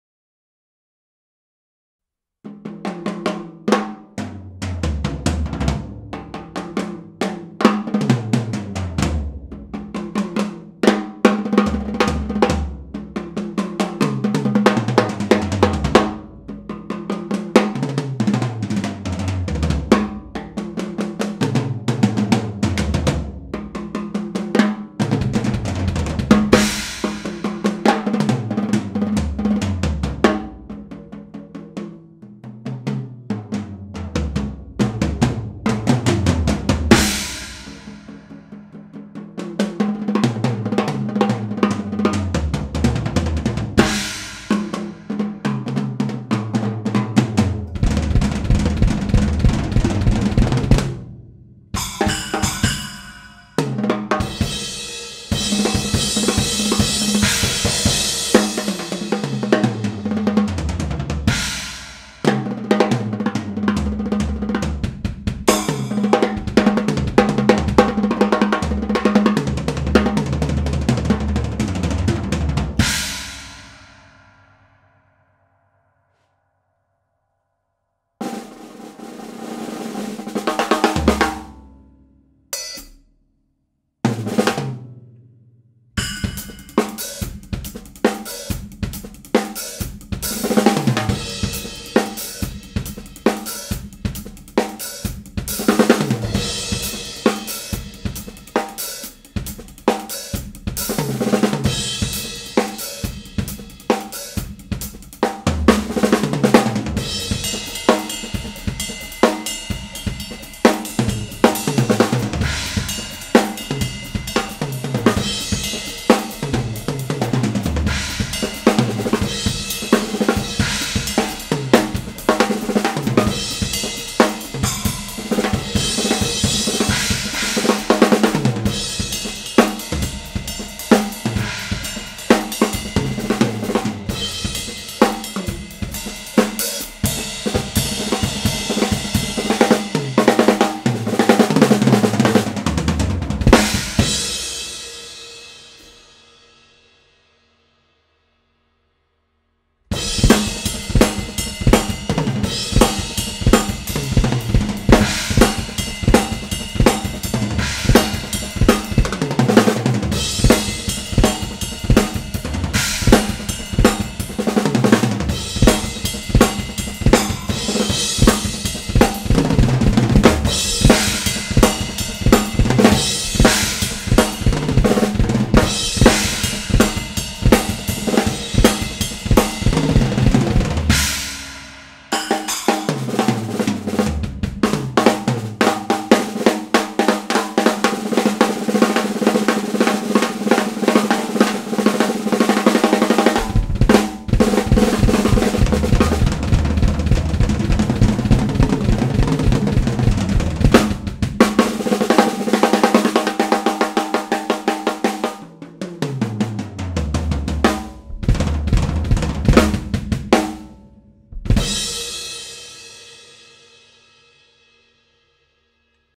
Miha Horvat - Drum Solo (Soundcheck)
My new video, a drum solo, improvisation. Recorded with 2 kick mics and 3 overheads.